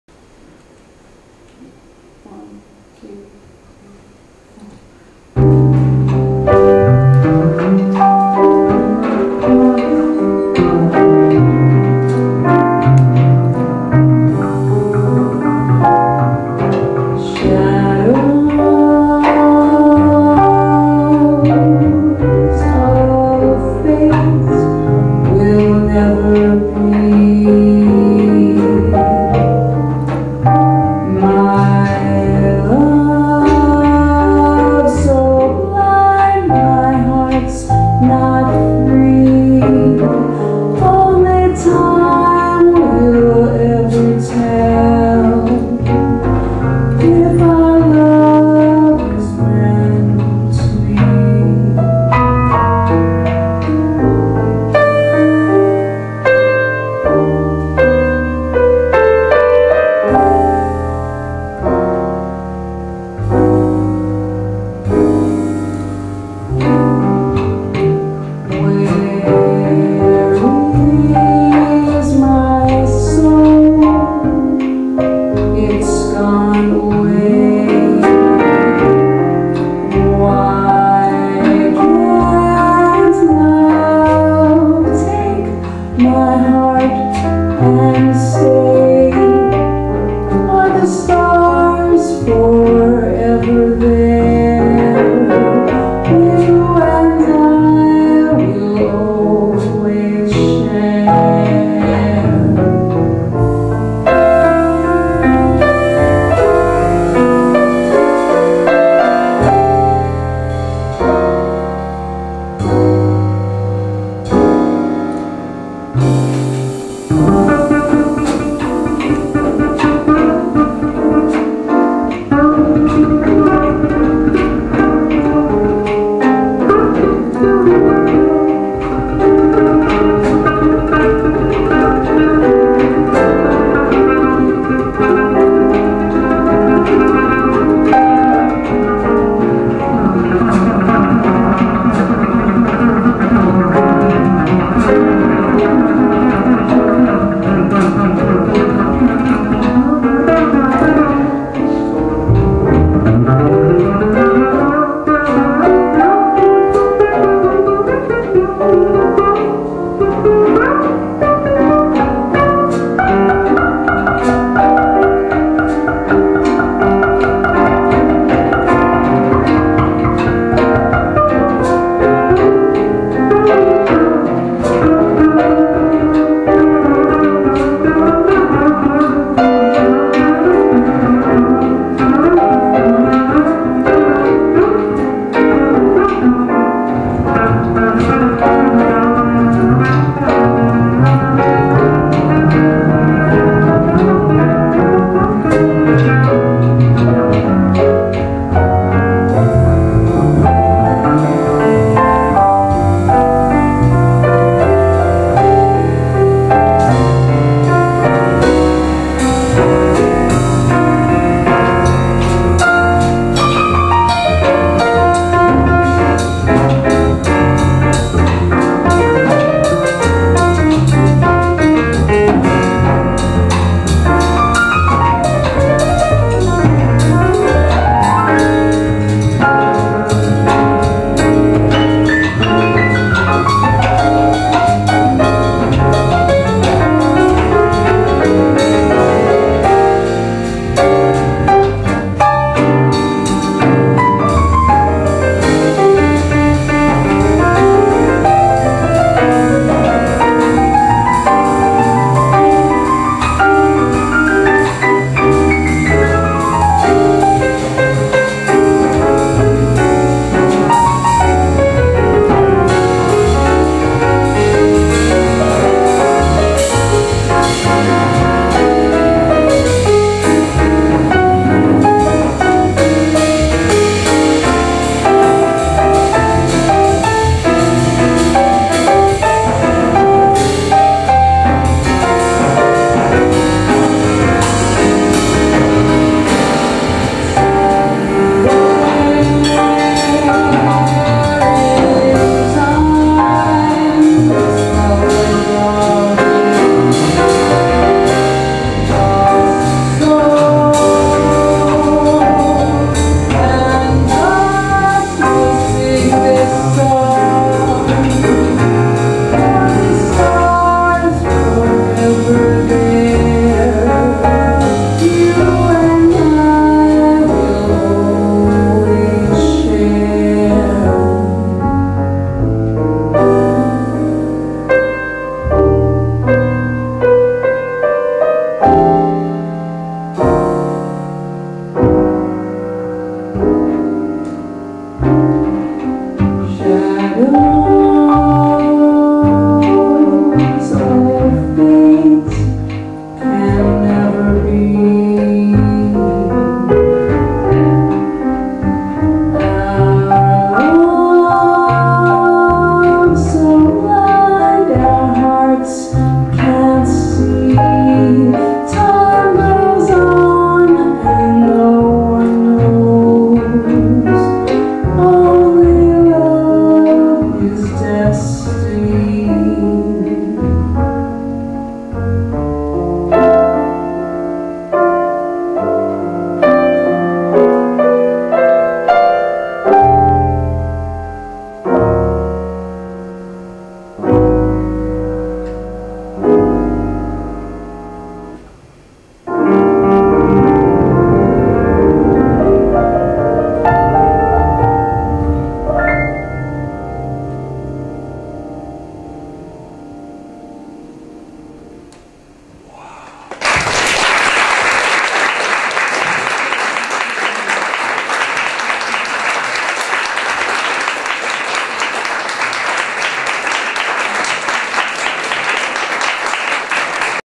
Roberta Piket Trio Live at Soka University
Roberta Piket Trio featuring Ratzo Harris on bass, Billy Mintz on drums and the leader on piano and vocals.